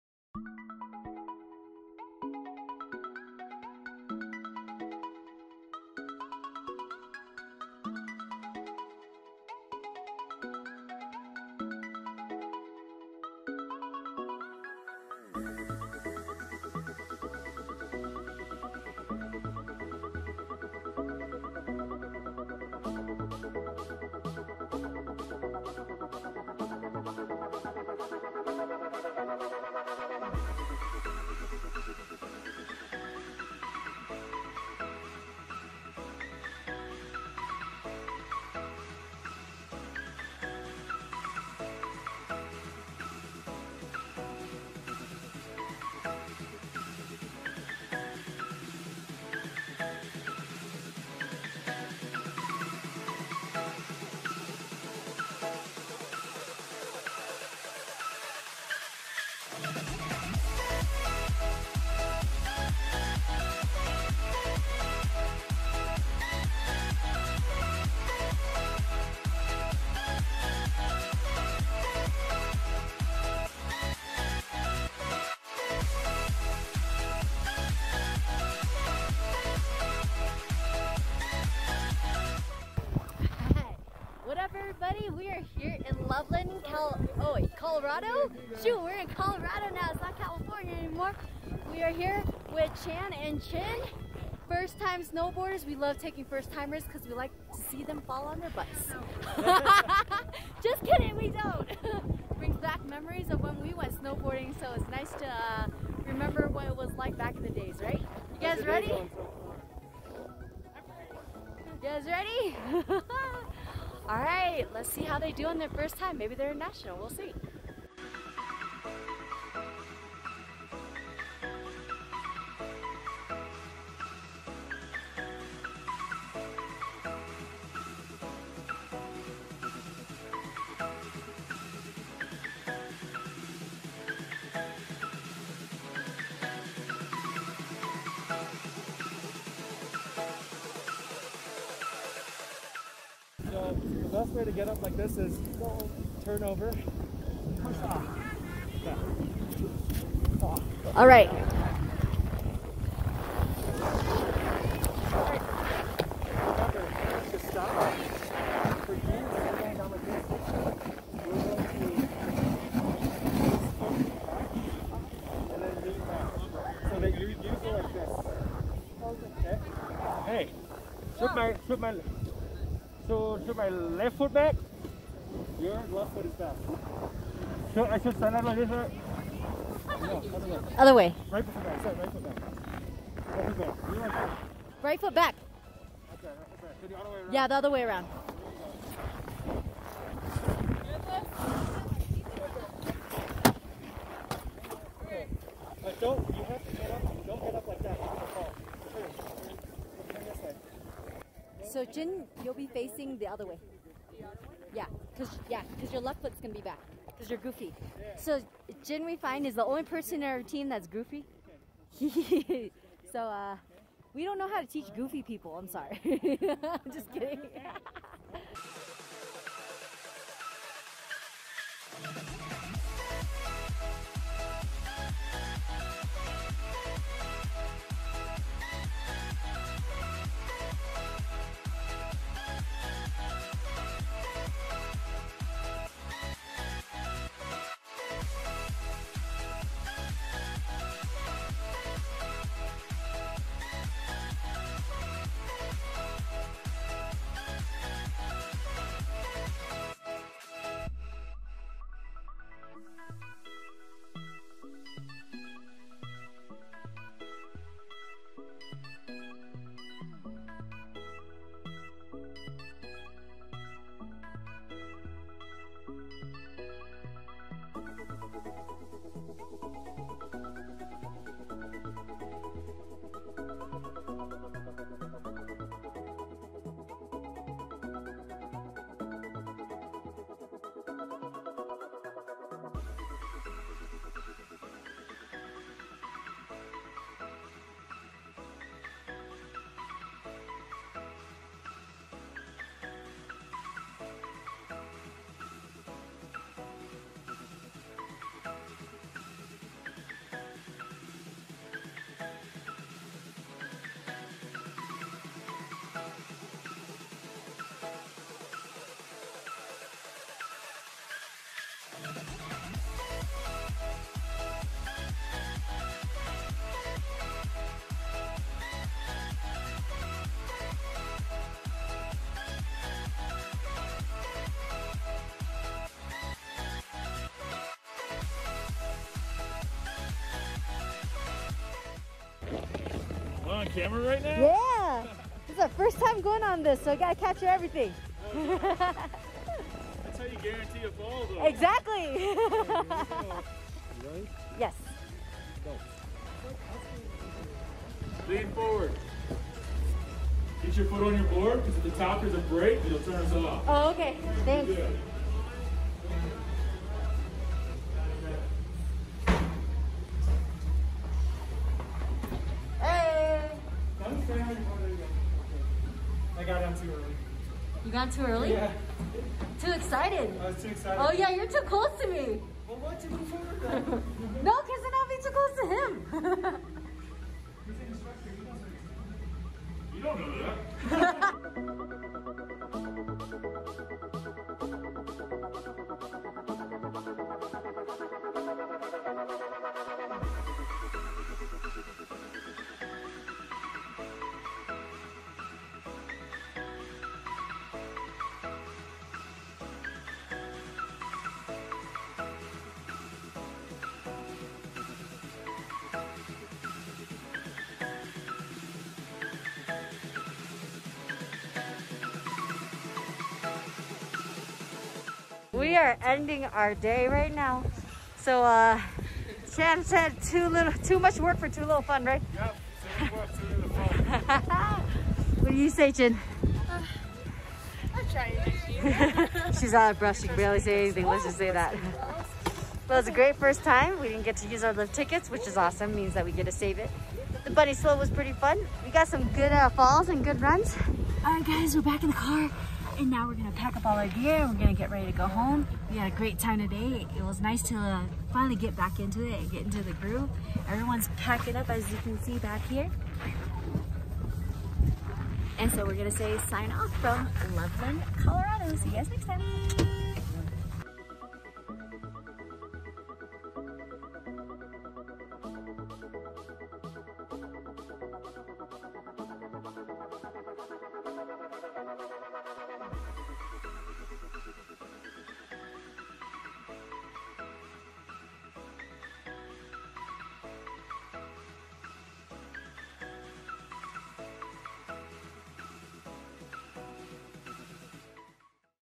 1st Time Snowboarding in Colorado 2020
1st touch of SNOW in Colorado! Teaching our cousins to snowboarding. Lots of fun and falling.